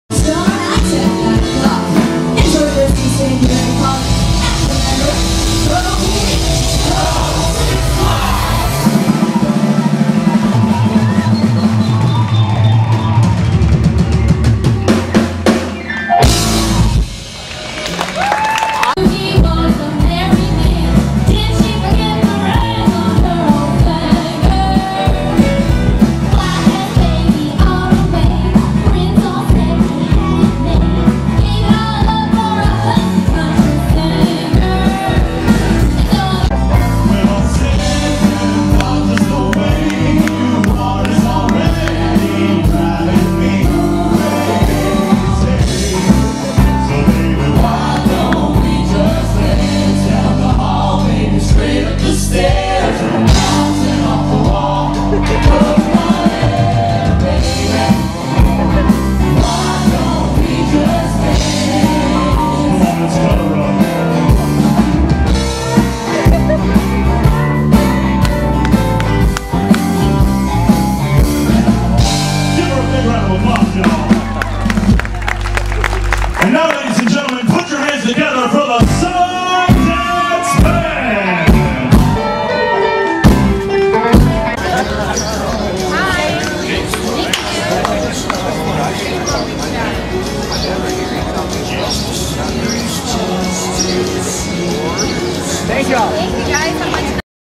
140615 Staesha Gentry On Stage FIESTA TEXAS Sing/Dance with Cowboy
We were watching a very good and entertaining country/western group on the 'Roadhouse Country' stage at Six Flags, Fiesta Texas when one of the cowboy singers came down and picked Staesha out of the crowd to sing and dance on stage with him. Just on a whim we decided to catch the show and it became one of the biggest highlights of our vacation in San Antonio! I don't know your name, young man, but thank you for making my daughter's day!